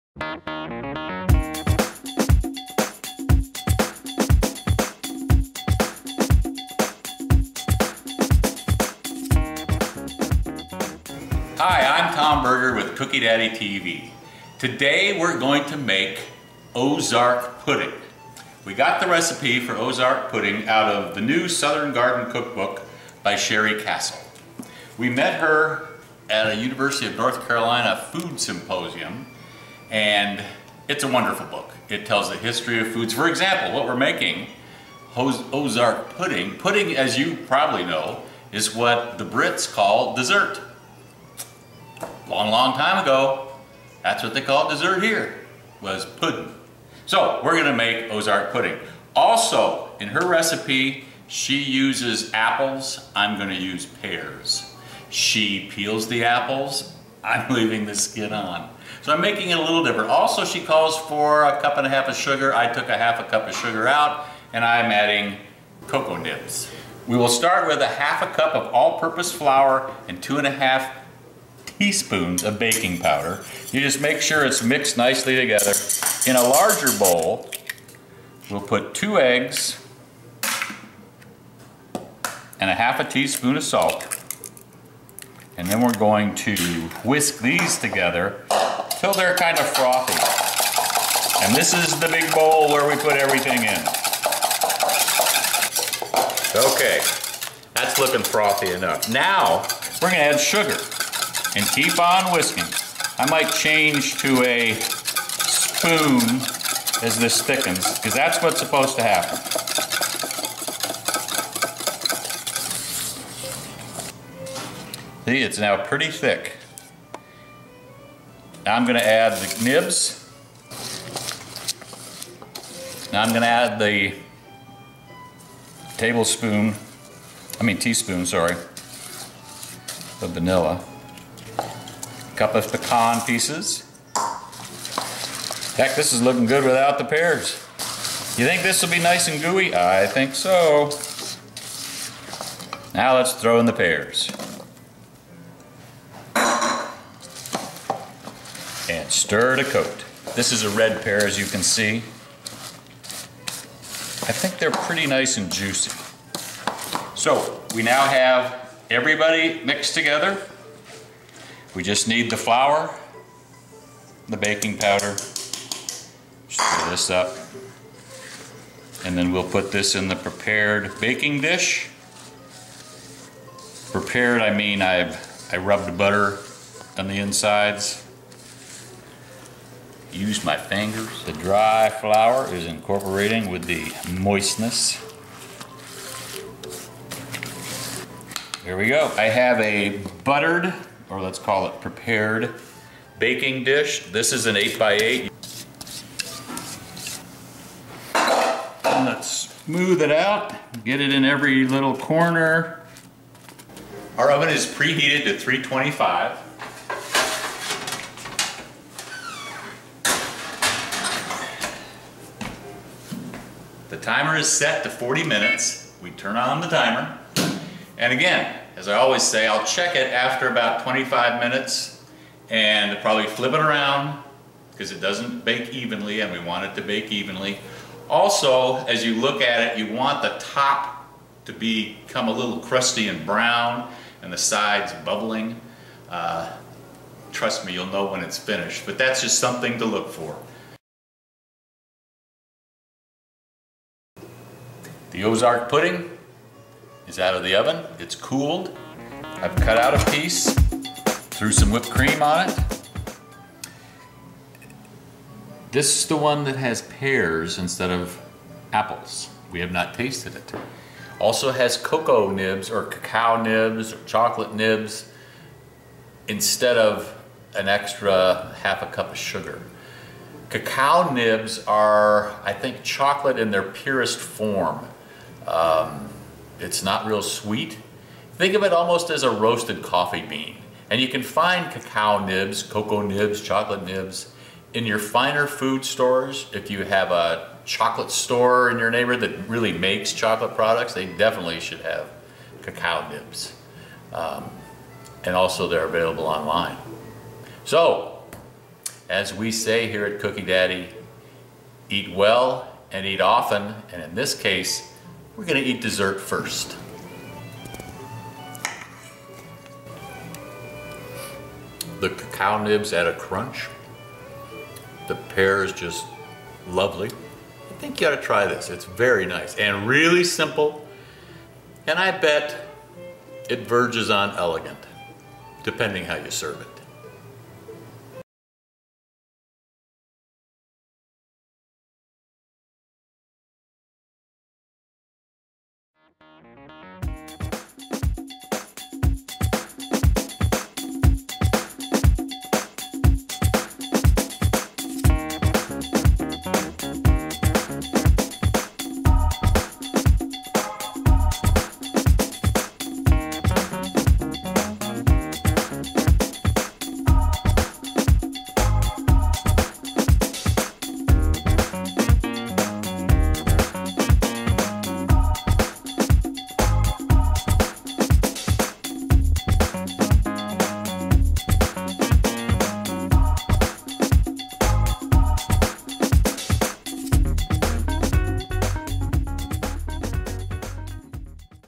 Ozark Pudding...Easy, Elegant
Ozark Pudding is not pudding in the Jell-O sense, but pudding in the English dessert sense. It's a mashup of parts of some of our favorite after dinner sweets, filled with fruit, nuts, chocolate and creating a toffee-like topping, as it bakes that you'll love. Don't hesitate to try your own variations on this theme.